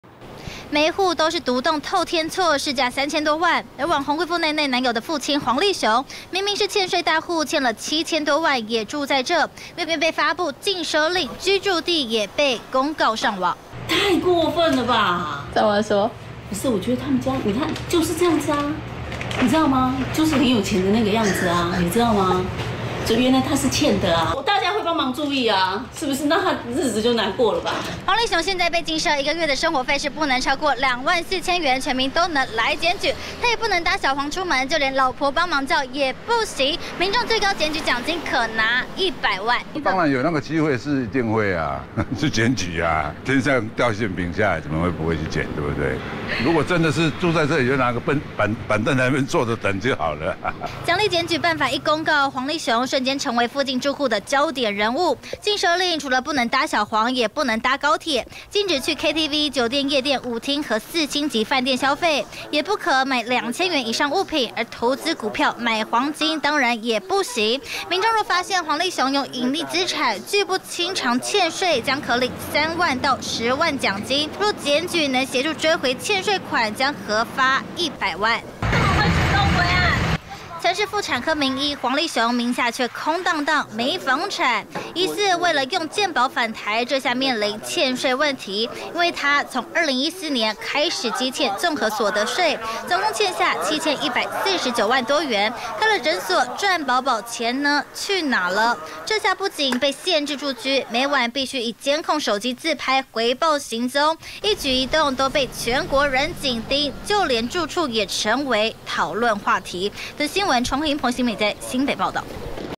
貴婦奈奈男友父被禁奢！ 居住地被公布「三千萬透天厝」@newsebc
網紅貴婦奈奈男友的父親、名醫黃立雄，欠稅高達7千多萬，遲遲沒有提出清償計畫，士林分署今天起正式將他列入「禁奢大戶」檢舉公告，居住地址也曝光，住在市價三千多萬的透天厝。民眾如果發現他違反「禁奢令」，像是，出門搭小黃、高鐵或進出高消費場所，都提出檢舉，最高能獲得100萬獎金。